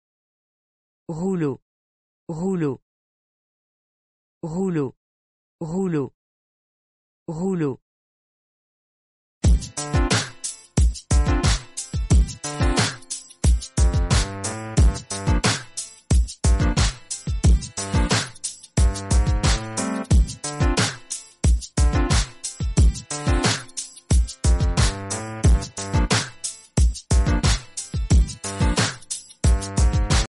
How do you say 'roll' in French?
How to say 'roll' in French? Listen to hear the pronunciation.